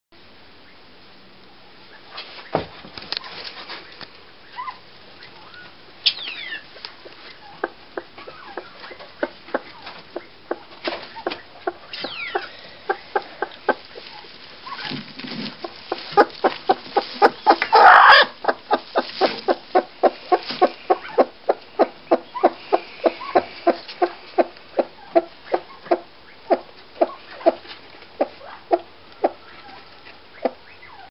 worlds best chicken!
this is by far the world best chicknen, the pheasant egg she was sitting on, she got off her nest and stole it from the other chichen that eats them after the pheasant lays them!